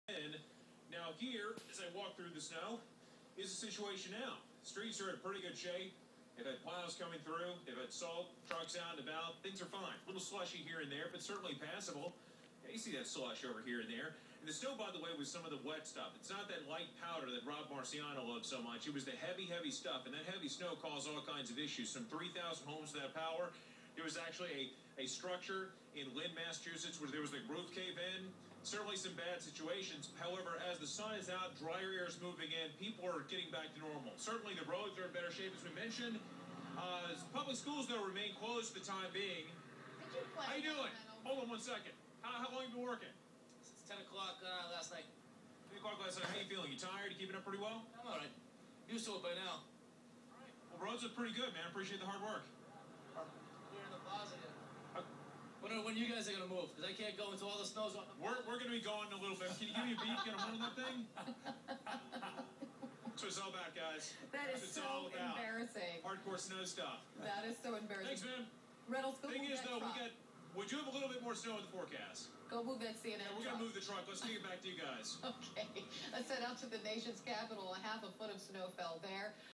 Snow worker asks CNN reporter to move!
It's funny how he really doesn't care that he's on TV with millions watching... He just wants to finish his job and get home!